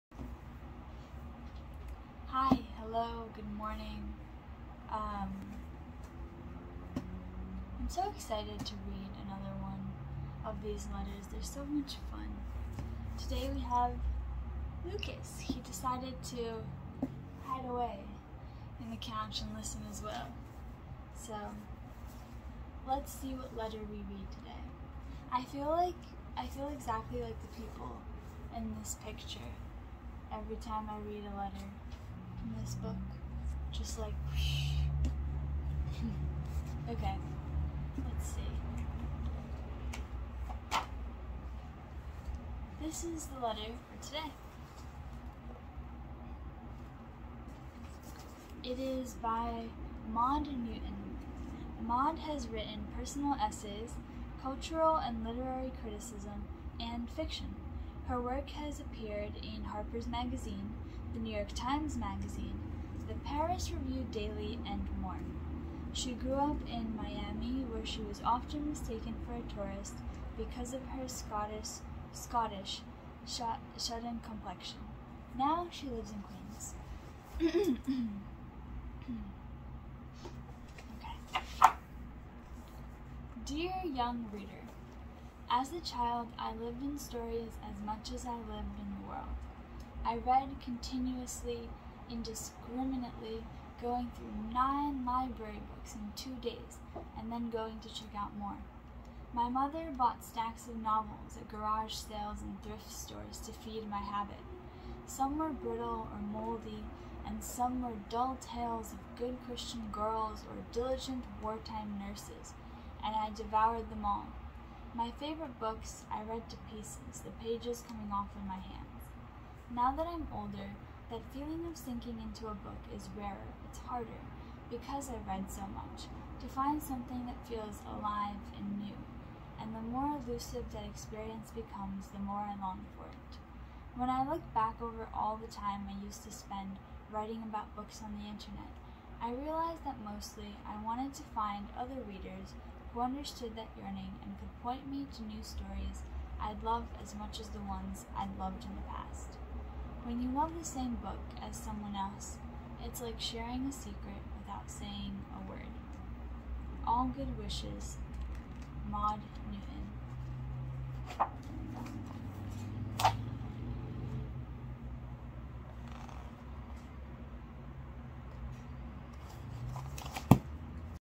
Day 9: A Letter to A Young Reader (Maud Newton)
A Velocity of Being: Reading a Letter to a Young Reader a Day

Maud Newton: written personal essays, cultural and literary criticism, and fiction

Artist of the image: Peter Brown